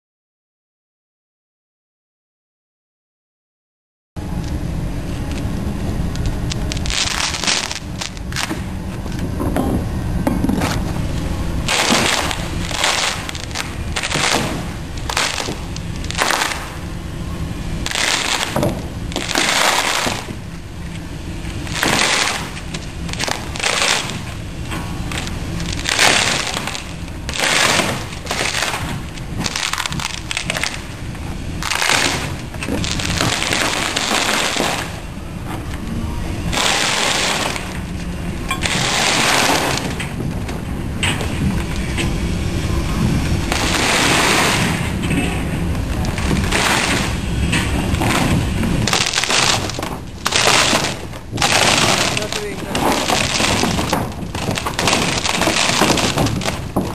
Compoflex 150 - Fagerdala - peeling of a plug for a boat hull
Easy peeling with Compoflex®
The Compoflex® microporous peel materials ensure very easy release when peeling while maintaining roughness. The bleeder layer collects any excess bleed material if applicable.

This video was recorded by Mr Hakan Johansson from Fagerdala Thiger Marine Systems, and shows the peeling of Compoflex 150 from a plug for a boat hull.